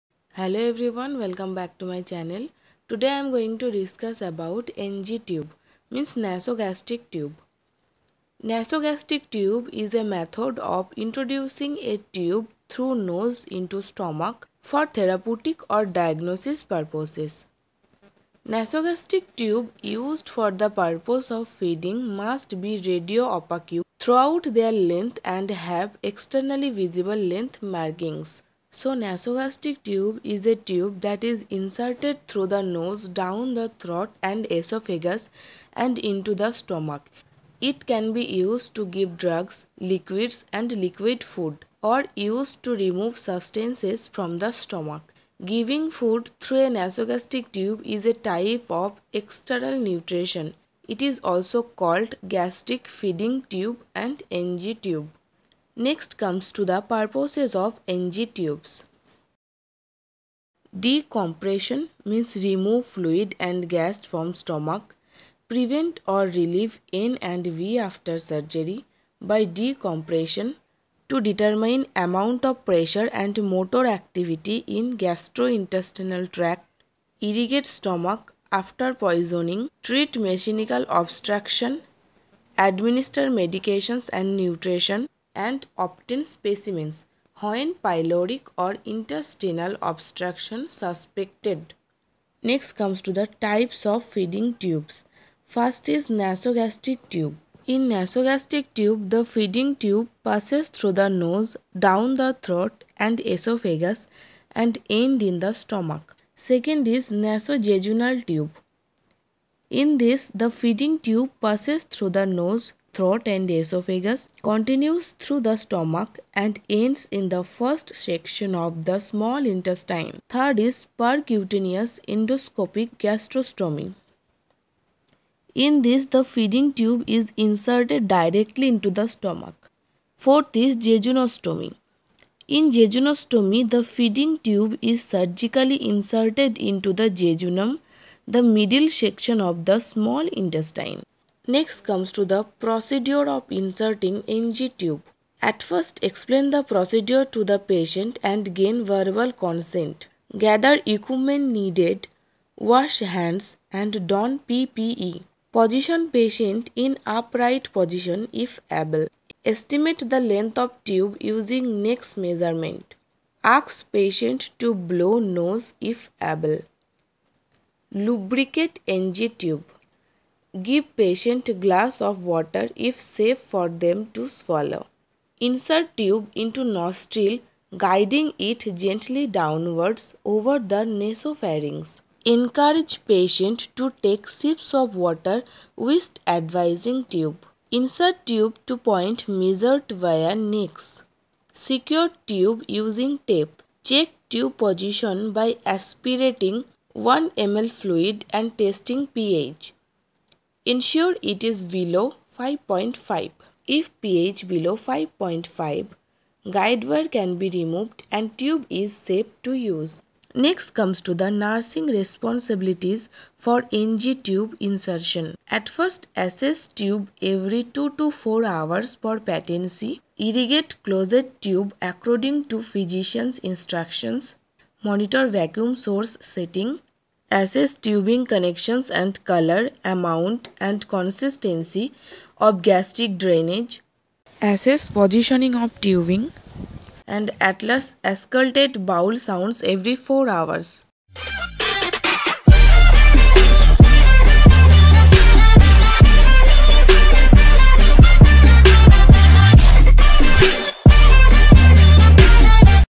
Nasogastric Tube || Fundamental Of Nursing
Nasogastric Tube
definition
purpose
Types
Procedure
Nurses Responsibility